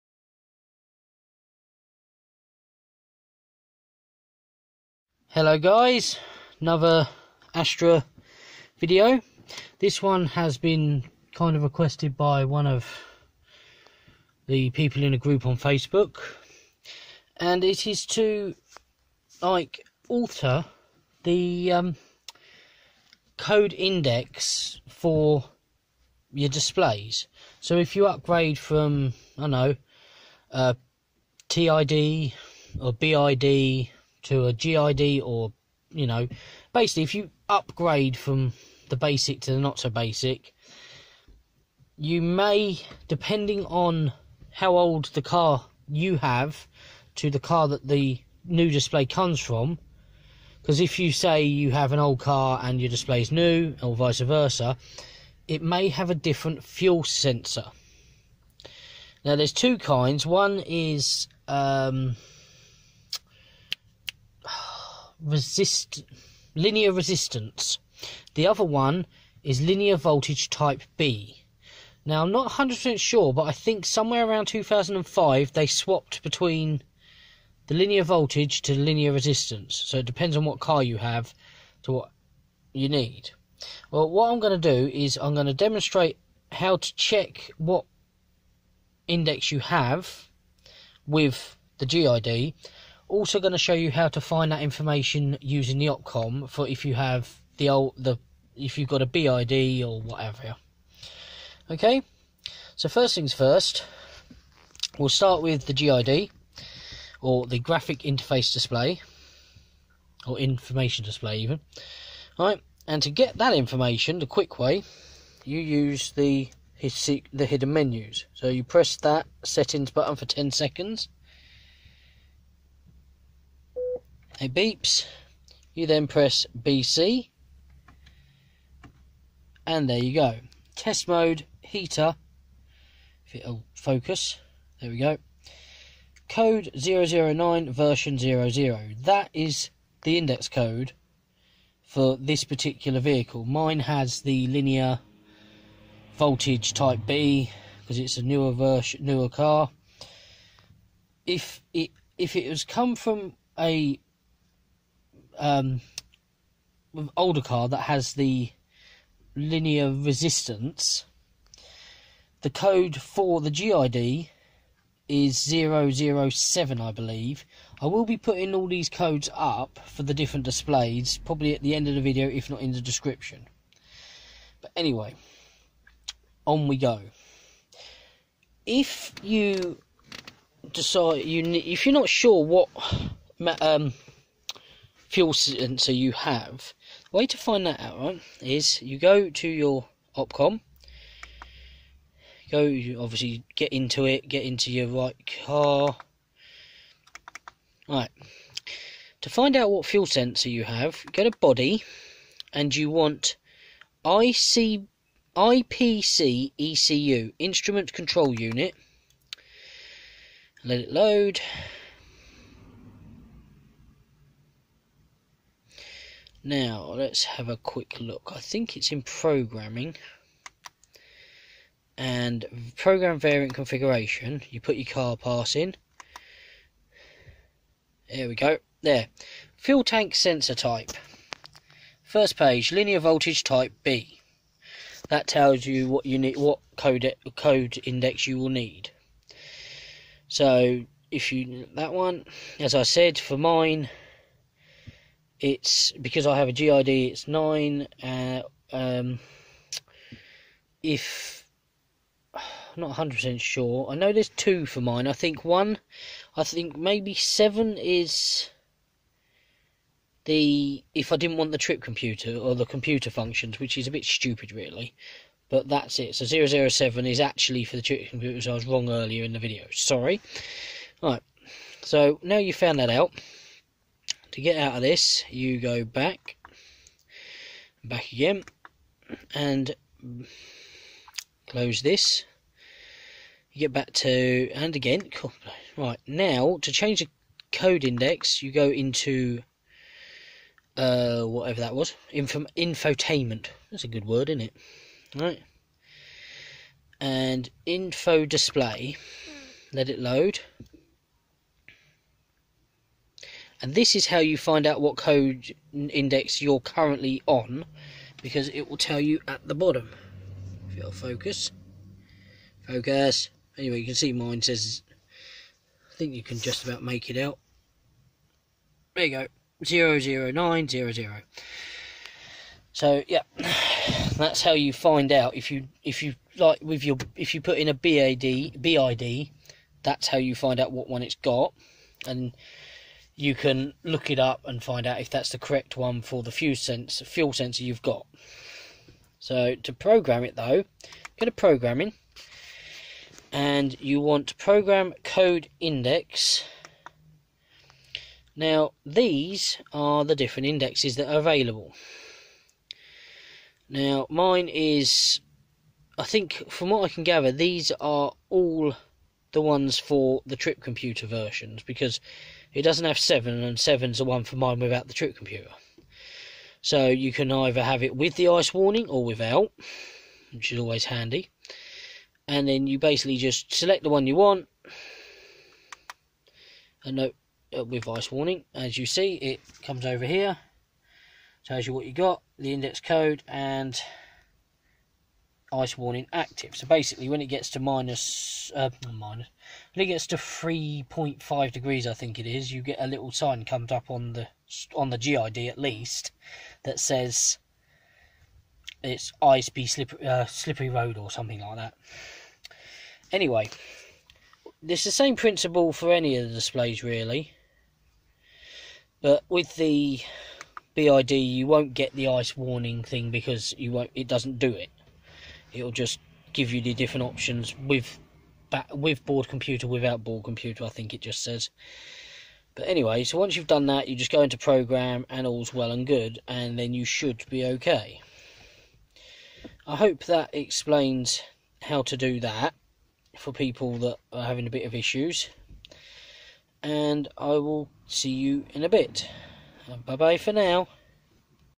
How to change code index on Astra H Infotainment displays
In this video I explain how to change the code index on Astra H infotainment displays

For Full list of the correct code index for the different sensors on each type of display please click on this